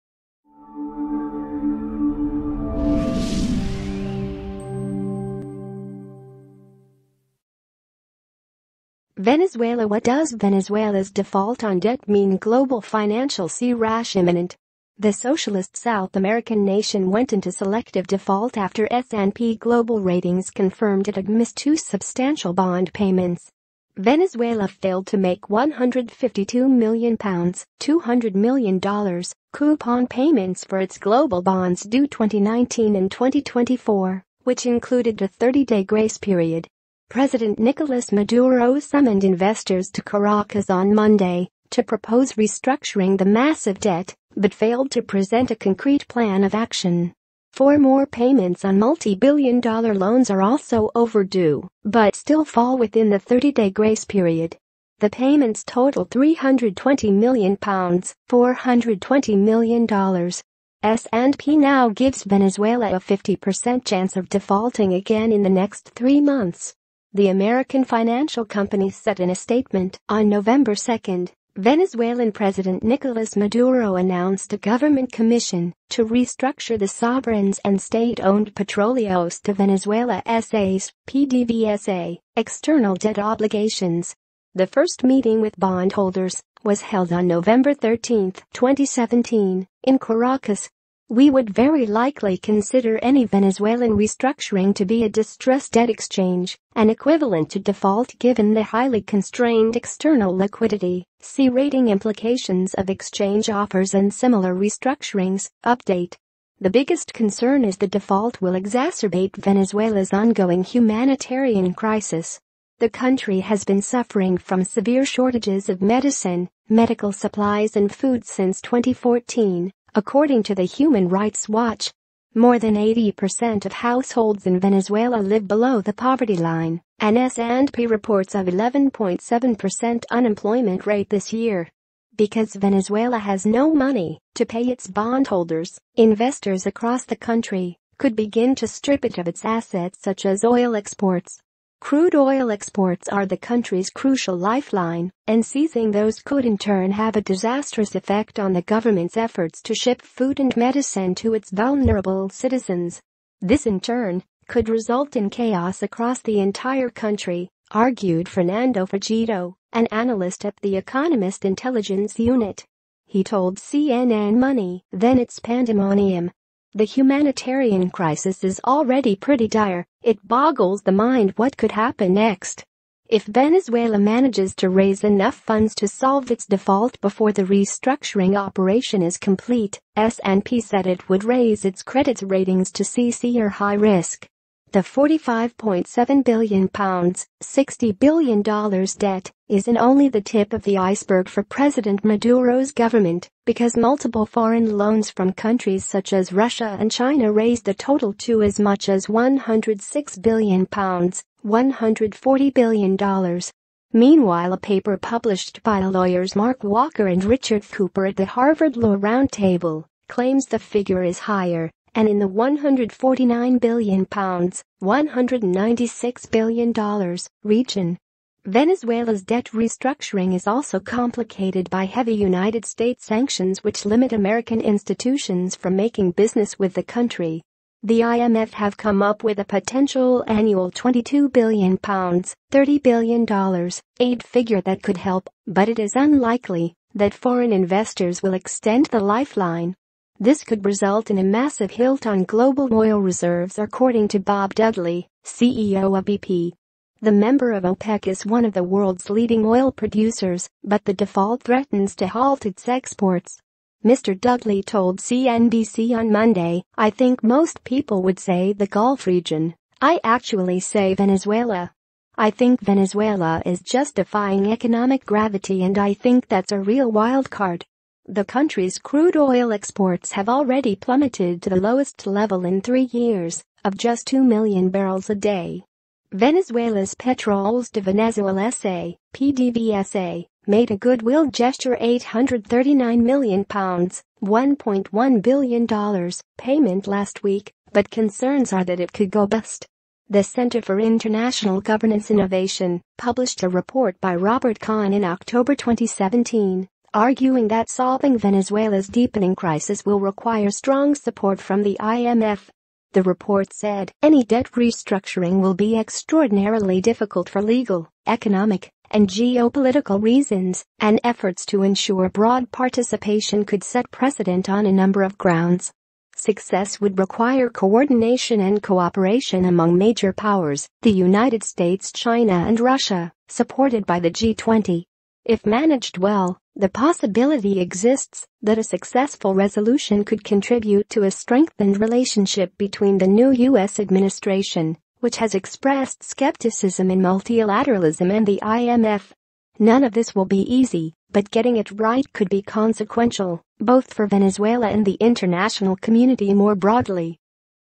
Venezuela What does Venezuela's default on debt mean Global financial crash imminent
Venezuela What does Venezuela's default on debt mean Global financial c.rash imminent.
The socialist South American nation went into selective default after S&P Global Ratings confirmed it had missed two substantial bond payments.

Venezuela failed to make £152 million ($200 million) coupon payments for its global bonds due 2019 and 2024, which included a 30-day grace period.

President Nicolas Maduro summoned investors to Caracas on Monday to propose restructuring the massive debt, but failed to present a concrete plan of action.